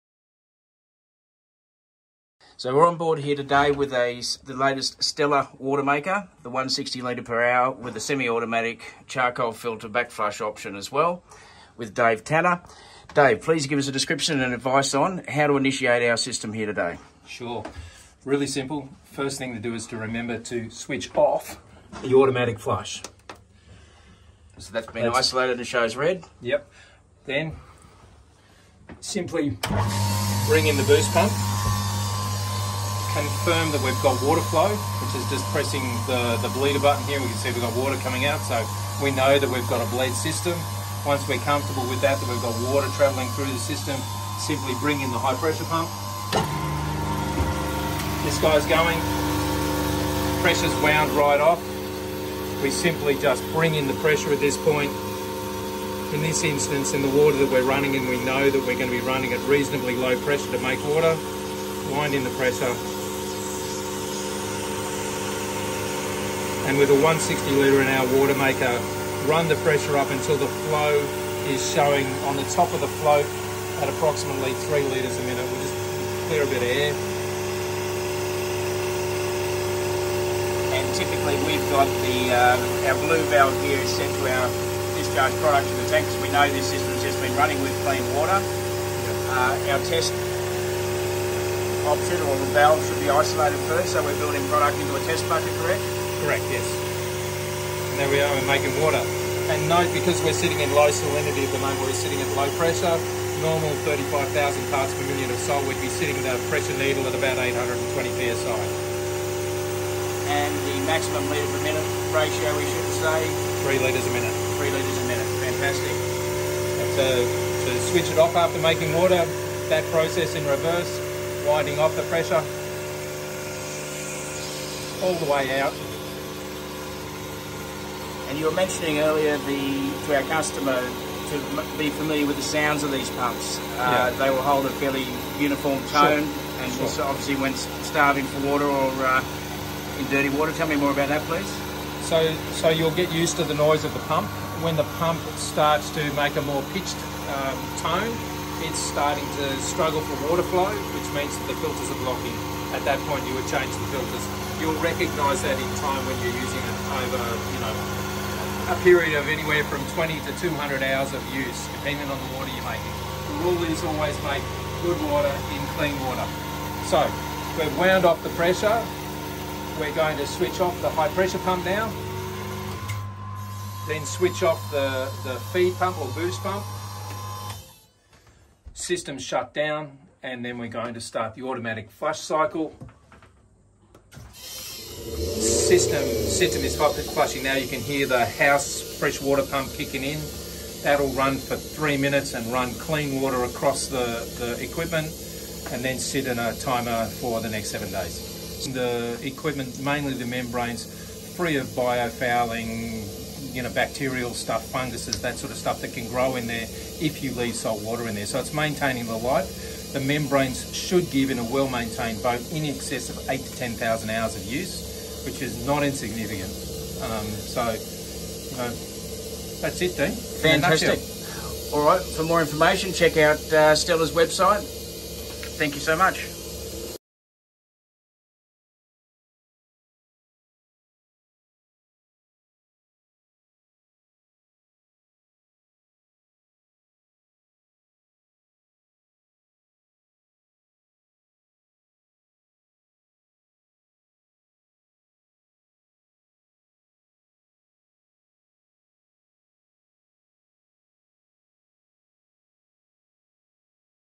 Stella “160 LPH” Water Maker operations video
Tutorial for 2022 model Stella Marine Systems “160LPH” Water Maker / Desalination System

This video is a guide only and isn’t a substitute to reading and understanding the operations manual.

Advice given is by Mr David Tanner & based on the 160 LPH unit.

Please contact Stella Marine Systems or visit their website